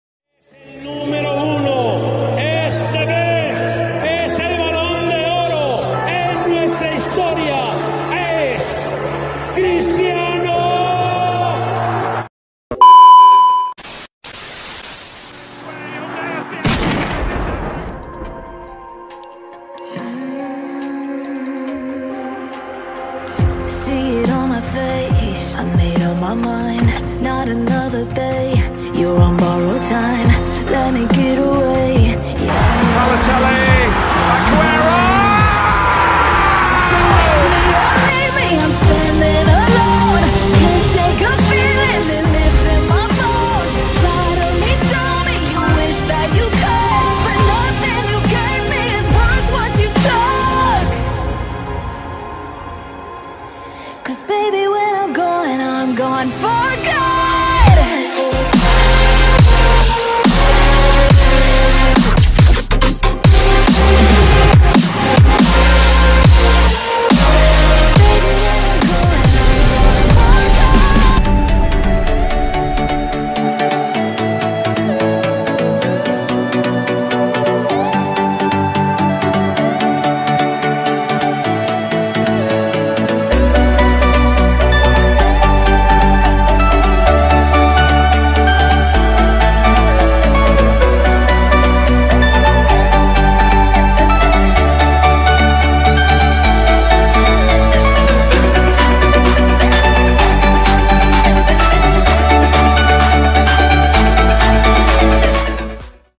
Sergio Aguero bergabung dengan Man City: Eksklusif di balik layar di Stadion Etihad
Debut aguero, Sergio Aguero bergabung dengan Man City: Eksklusif di balik layar di Stadion Etihad